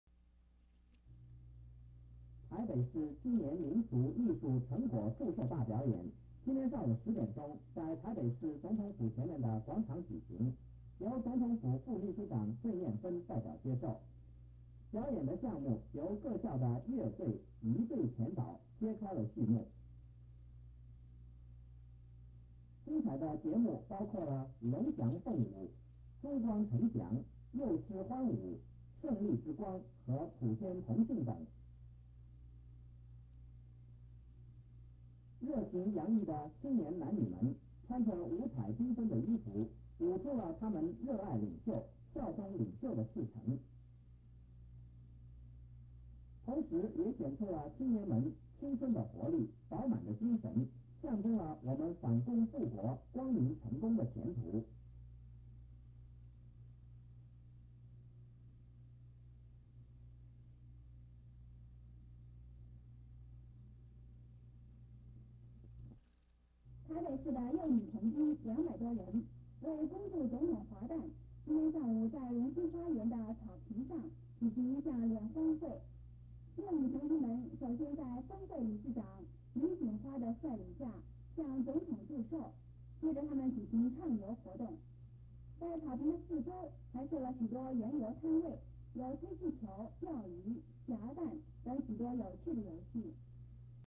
A.儀隊表演及青年民俗表演. B.女童軍聯歡會. (第一盤)
(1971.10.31)台北市青年民俗藝術成果表演在台北市總統府前之廣場舉行，由各校之樂儀隊前導揭開續幕，精彩節目包括龍飛鳳舞、普天同慶等，青年男女熱情洋溢舞出效忠領袖之赤誠；台北市幼年女童軍兩百多人，在呂錦花率領下，在榮星花園草坪上舉行唱遊活動，向總統祝壽。